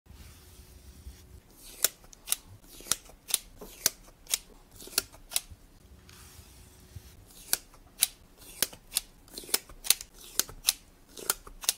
Would you try Cotton Candy Forbidden Ice Cream? 🤯
This Cotton Candy Sugar Scrub has fruity notes of strawberry & raspberry, laced with caramel swirls & hints of vanilla 🍬